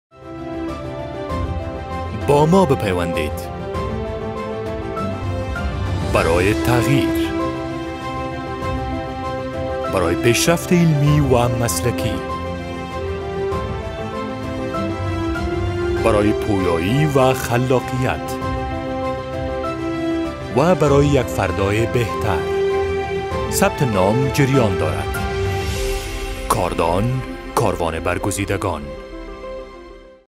Kardan University TV Commercial for Spring 2021
با ثبت نام در برنامه‌های تحصیلی پوهنتون کاردان در مسیر یک فردای بهتر گام بردارید. ما شما را با کاروانِ برگزیدگان در بهترین پوهنتون افغانستان وصل می‌کنیم.

برای آگاهی در مورد برنامه‌های تحصیلی ما: kardan.edu.af/admissions